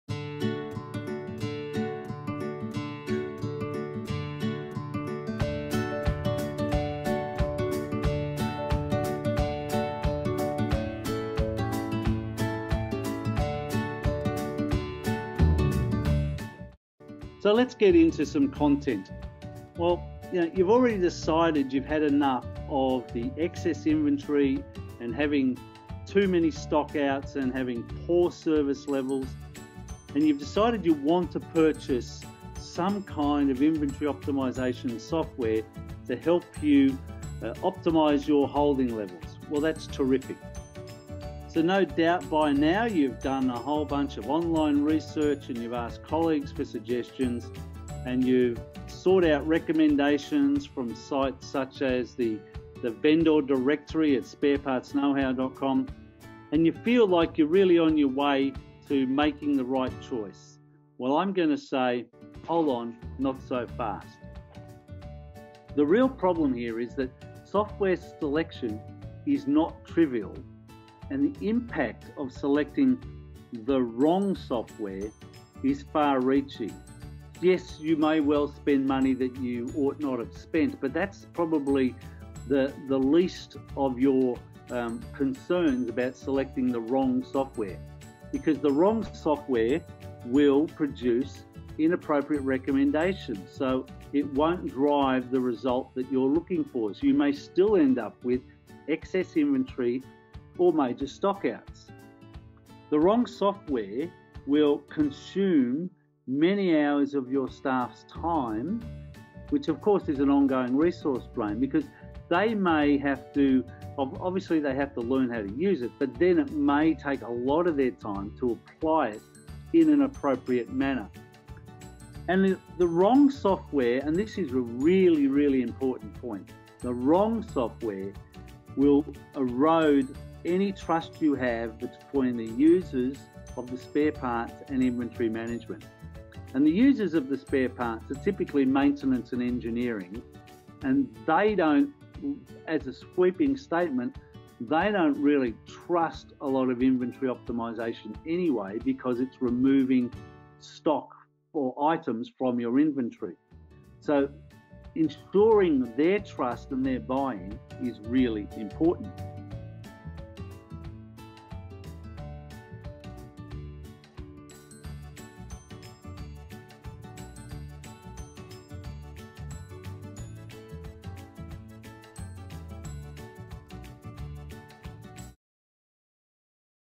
Optimization Software Selection is Not Trivial
What is the impact of selecting the wrong optimization software?

Not so wrong that it doesn’t work, but sufficiently wrong that the software doesn’t provide the best results possible?

How much would it cost your company if the results are ‘good enough’ but not as good as they should be?
If ‘getting a better number’ really the goal?

The result could be inappropriate recommendations, waste of hours (months?) of staff time, and the erosion of trust with the end users.

This is explained in this short video, an edited extract from a live webinar titled: Selecting the Right Spare Parts Inventory Optimization Software for Your Company